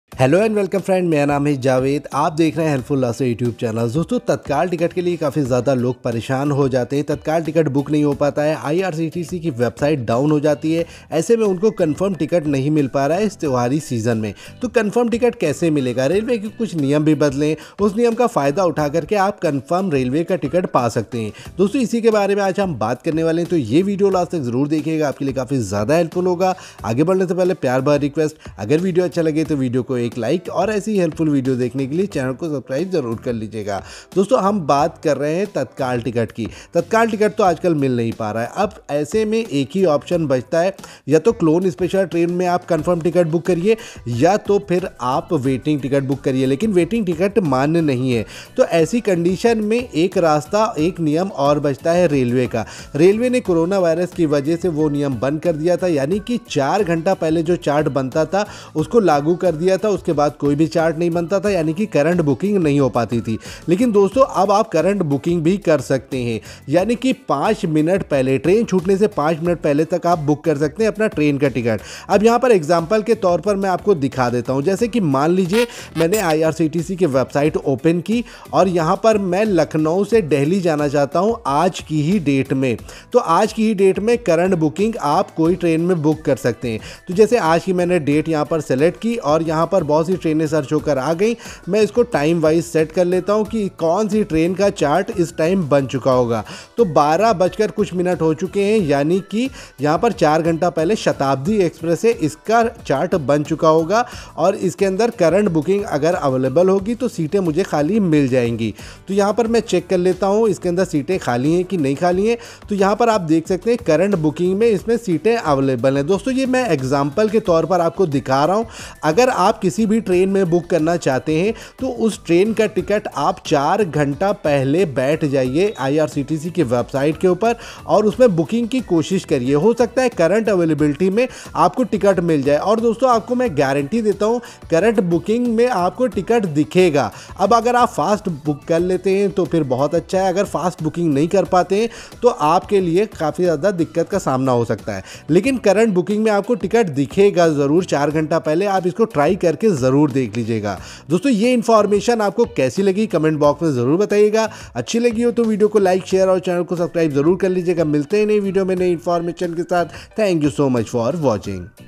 How To Book Confirm Train Ticket Online On Irctc Website And App Without Including Tatkal Quota.
HELLO AND WELCOME FRIENDS HELPFULL RASTA CHANNEL ME APKA BAHOT BAHOT SWAGAT KARTA HU DOSTO AAJ MAI APKO BATAUGA IRCTC TRAIN TICKET BINA TATKAL KE KAISE KAR SAKTE HAI
AGAR APKO CONFIRM TRAIN TICKET NAHI MIL PA RAHA HAI TO APKO RAILWAY KA NAYA NIYAM USKA FAYDA UTHANA CHAHIYE BINA TATKAL KE AAP CURRENT BOOKING KAR SAKTE HAI TRAIN CHARRT BANNE KE BAAD TRAIN KA CHART 4 HRS PAHLE BANTA HAI WAHI TIME APKO BOOKING KARNA HAI TRAIN ME APKO CONFIRM SEAT MIL JAYGI OK DOSTO IS VIDEO ME AGAR KUCH NA SAMJH AYA HO TO AAP COMMENT ME JA KAR PUCH SAKTE HAI AUR VIDEO KO LIKE KAREN AUR HELPFULL RASTA KO SUBSCRIBE ZARUR KARE THANK YOU FOR WATCHING.

@@@@@@@@@@@HELPFULL RASTA@@@@@@@@@@@